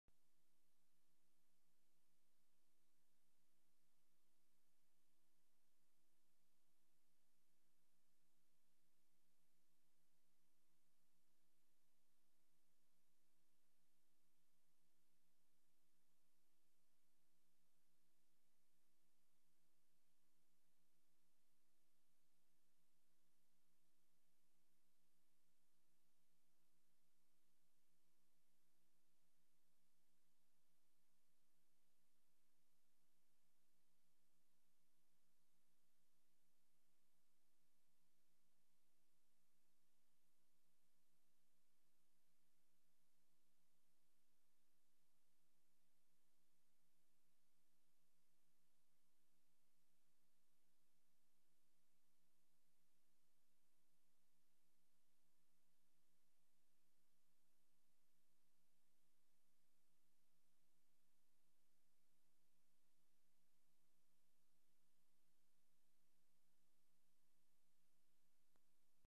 JAMBOREE - NO SOUND
People seated on ground. Crown Prince Constantine arrives with Princess Anne-Marie. Greek priests. Crowds. Speeches. Boy Scouts. Rockets bursting in the sky.